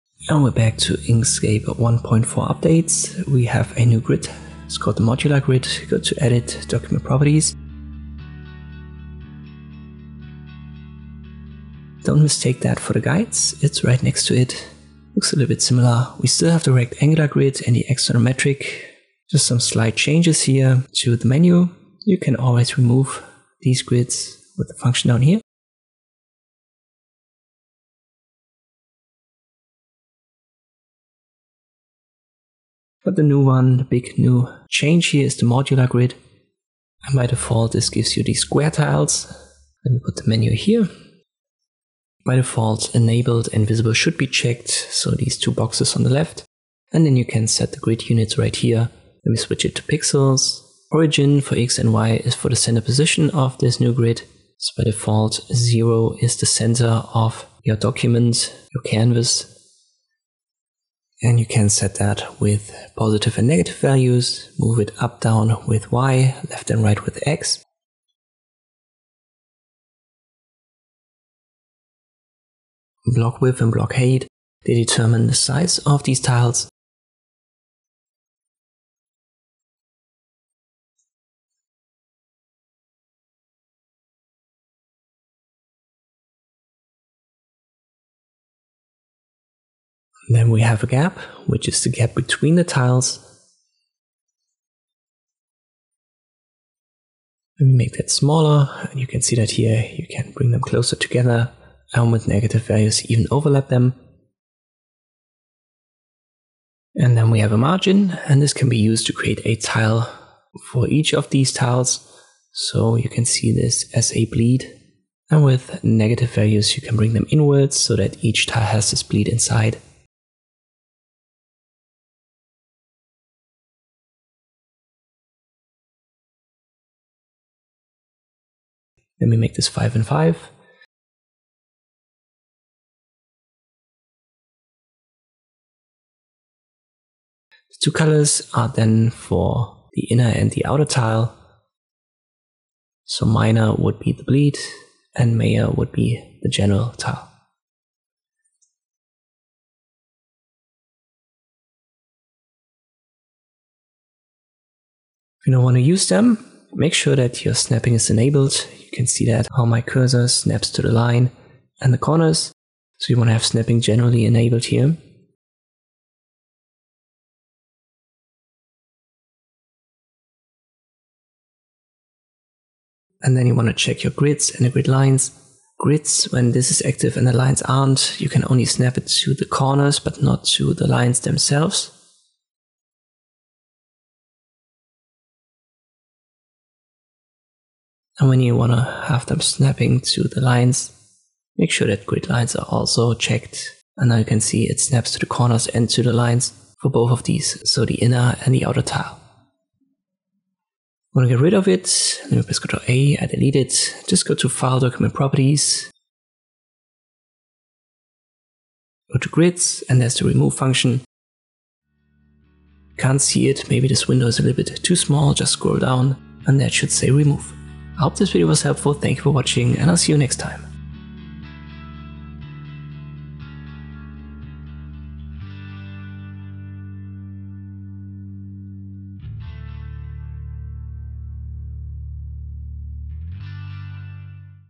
New Modular Grid in Inkscape 1.4
In this tutorial I am going to show you the new Modular Grid in Inkscape.